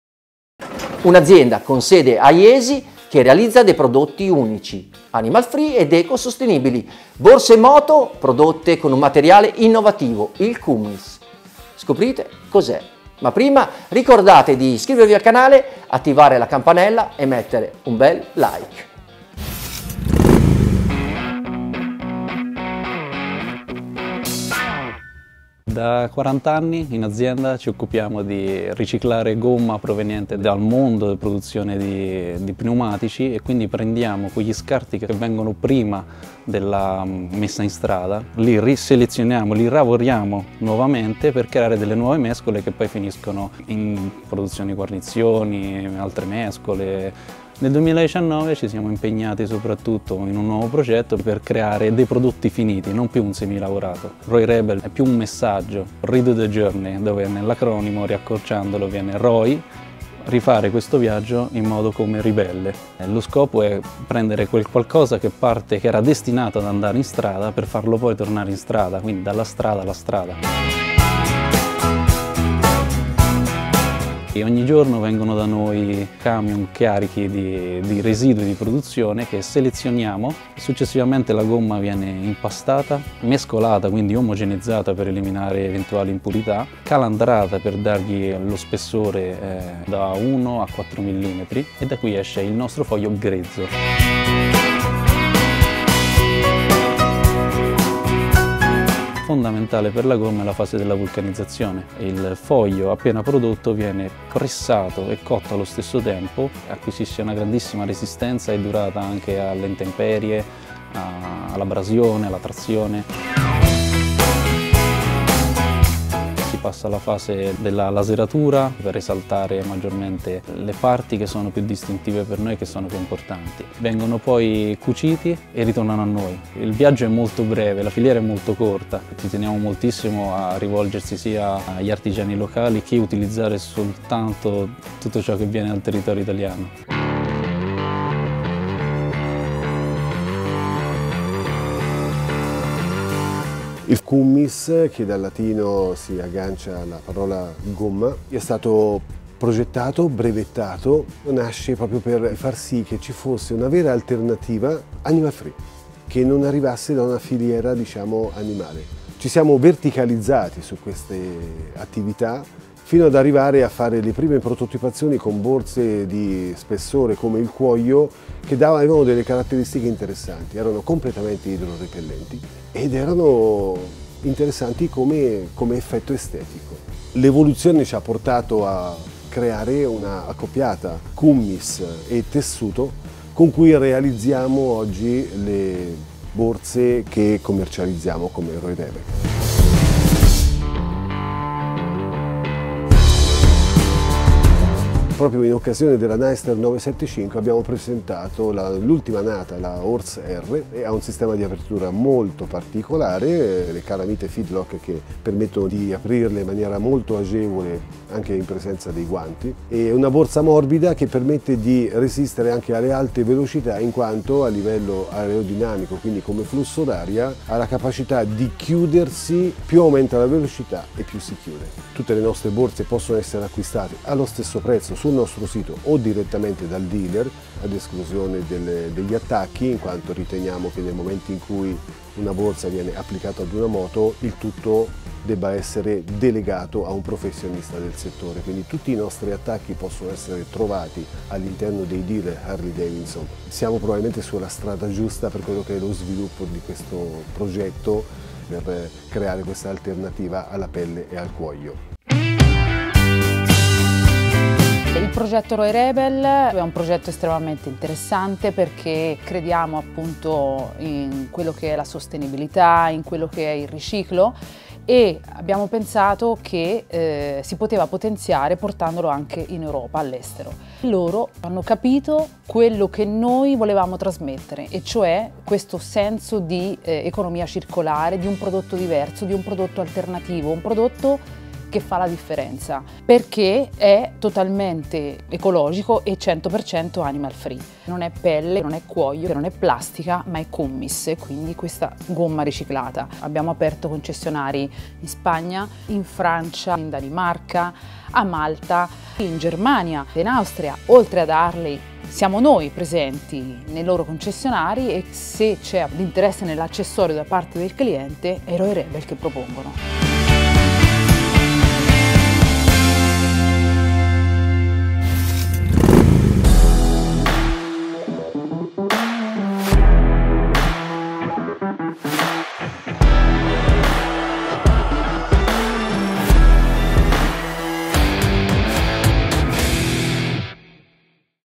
DCM - Roy Rebel
Un’azienda con sede a Jesi che propone dei prodotti unici sul mercato, 100% animal free, ecosostenibili e indistruttibili.
Borse moto in primis ma anche scarpe, stivali, zaini realizzati da Roy Rebel con il Cummis, un materiale innovativo ricavato dai residui di produzione degli pneumatici.